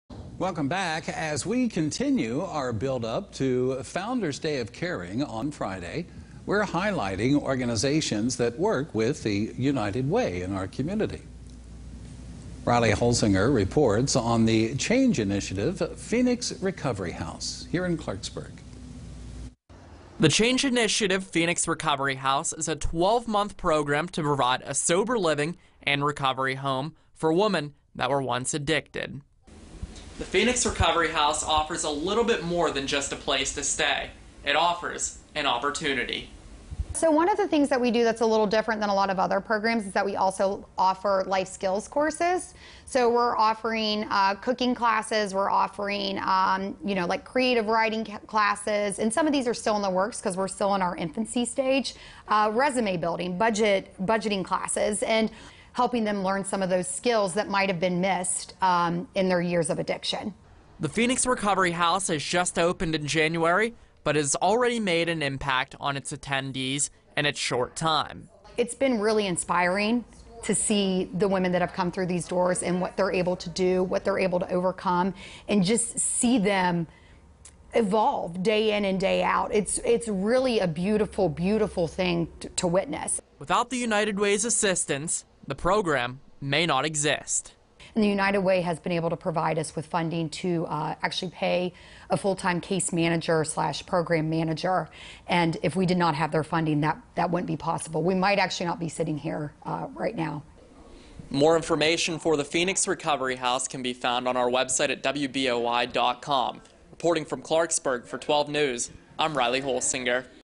Founder's Day of Caring: Phoenix Recovery House
One of the United Way's partner agencies, Phoenix Recovery House, helps women who have struggled with addiction get back on their feet.